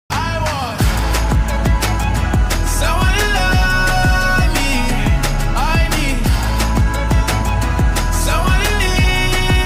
Driving over the Lions Gate Bridge in Vancouver
Ever wondered what it's like to drive over the Lions Gate Bridge in Vancouver?